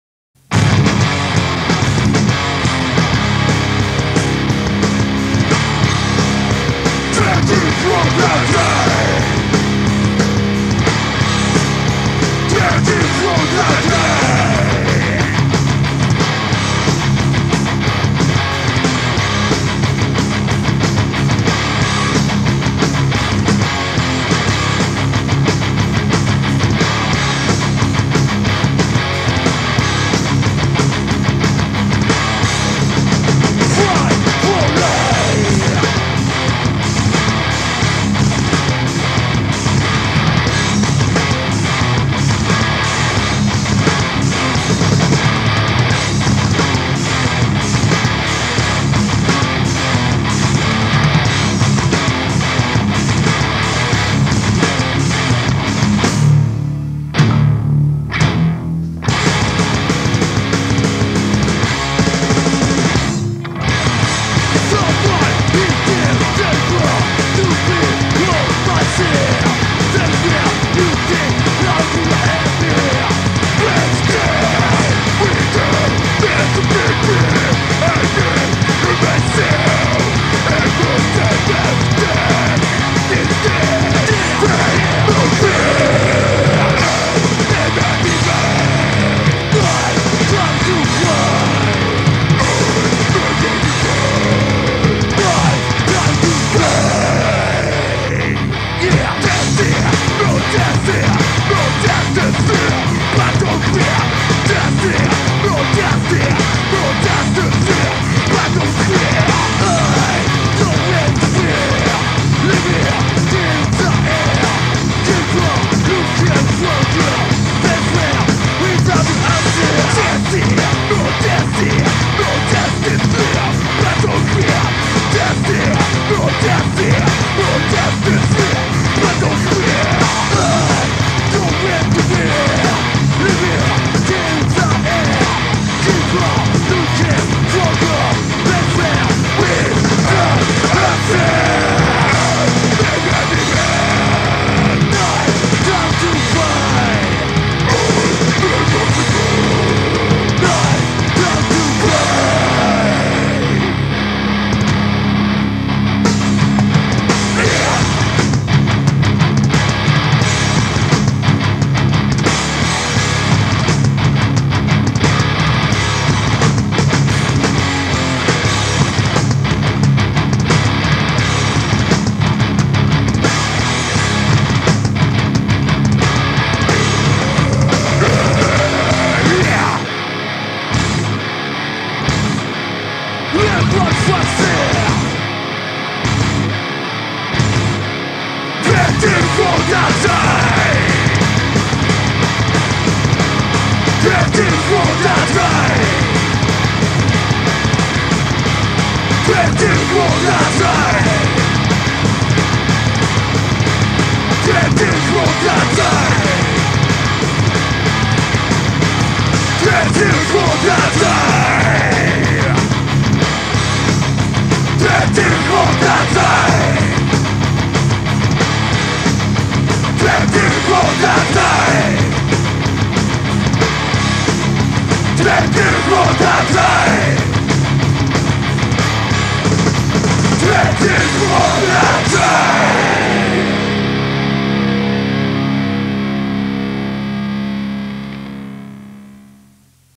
キャプテンNUNOのデモテープ紀行＃06「24TRIBE」
キャプテンが貯蔵するデモテープを勝手にアップしていきます。
音質はデモテープなんで、、、

第六弾は大阪のNxYxHxCバンド
「24TRIBE」
聴いた瞬間ワルを感じさせてくれる極悪サウンド。
ツインボーカルが叩き込む迫力とタフネスが存分に堪能できる一曲。
現在は解散しています。